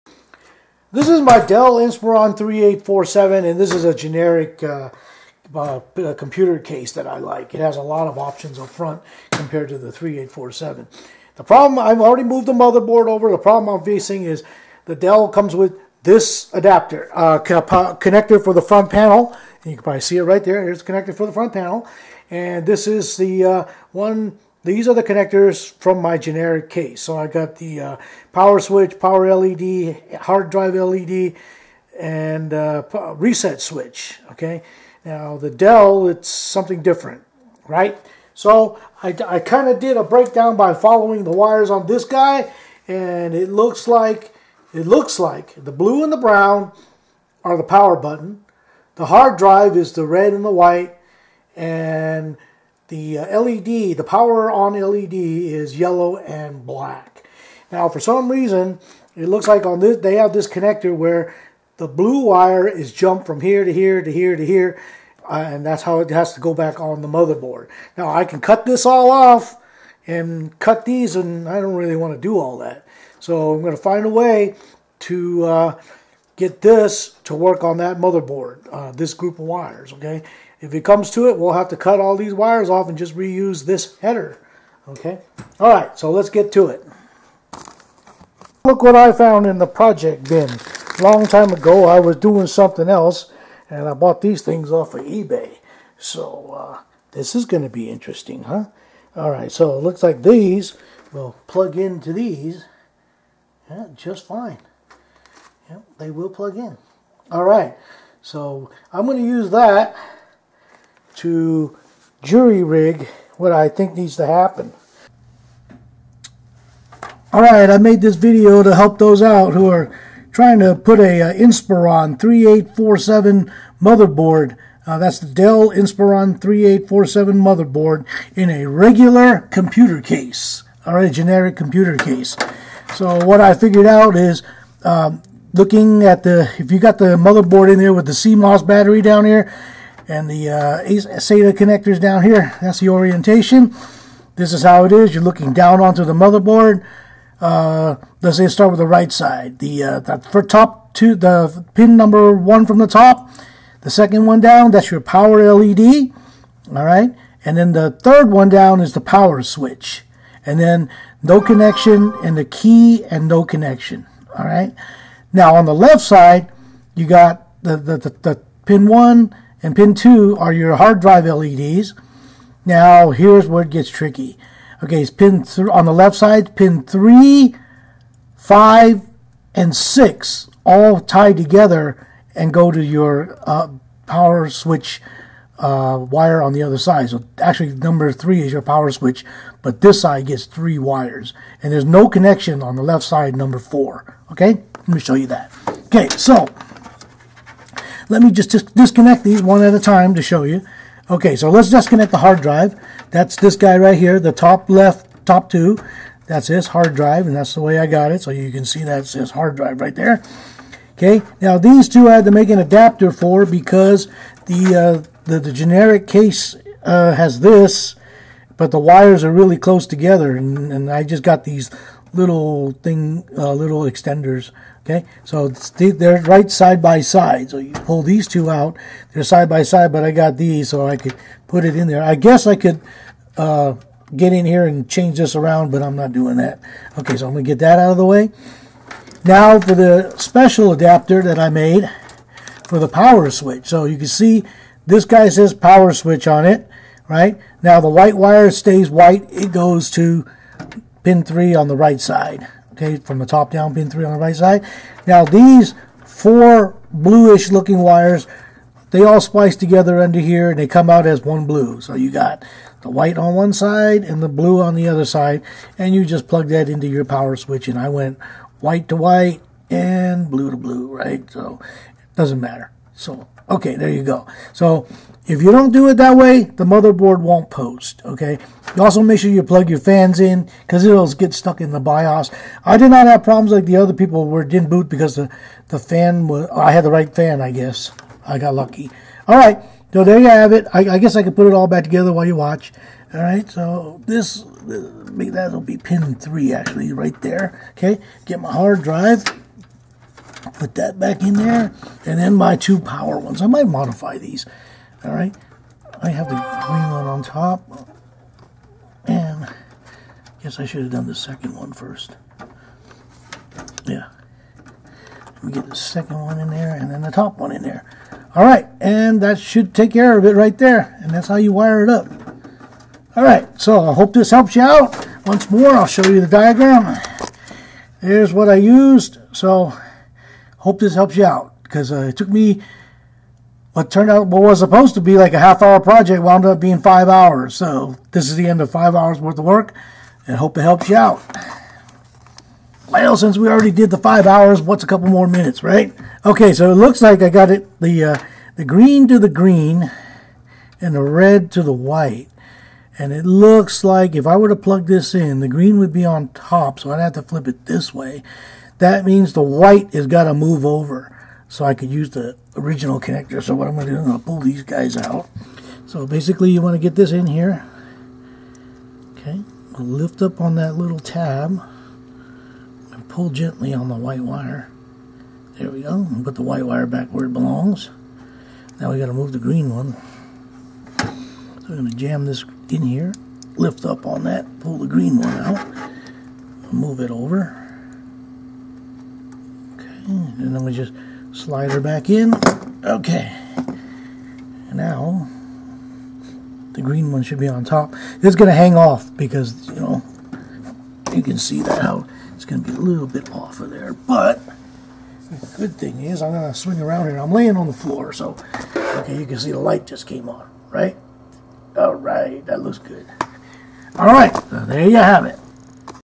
Dell Inspiron 3847 Front Panel Panel Header Mod for non Dell case.
Front panel header pins decoded and modification.